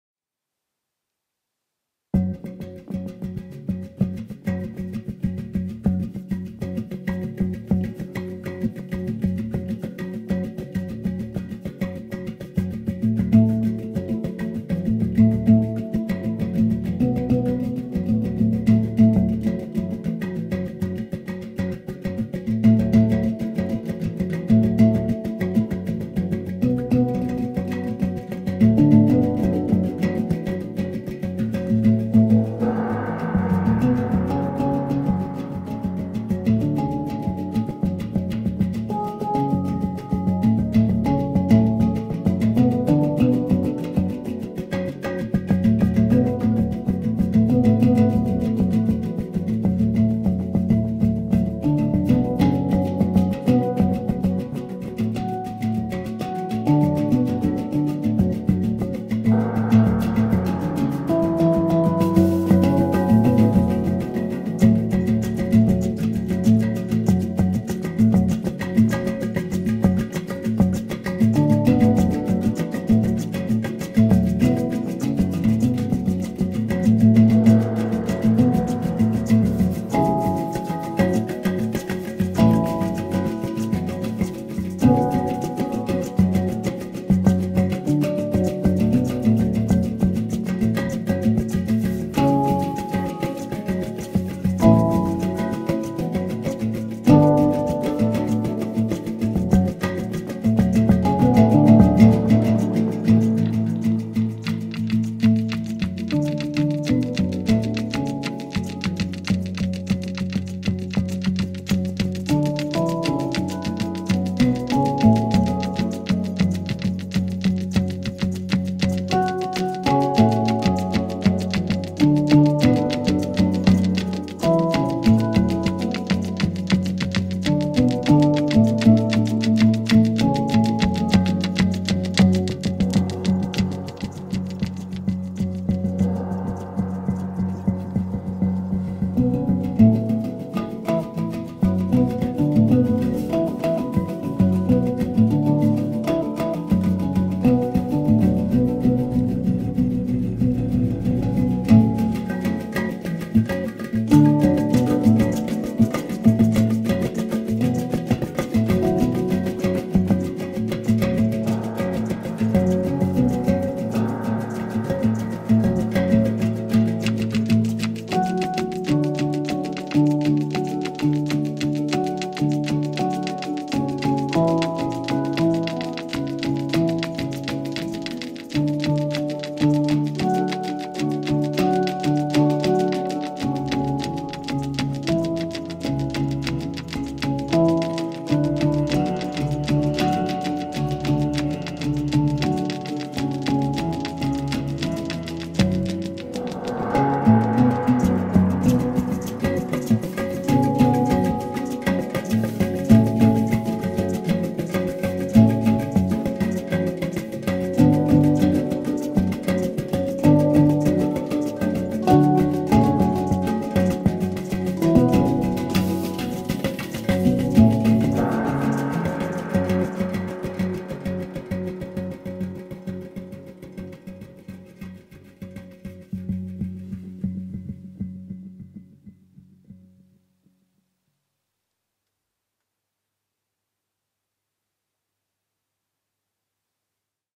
Pablo La Porta plays Hang ,cymbal ,stones and delays .
Pablo La Porta plays Hang Panart ,cymbal ,stones and delays .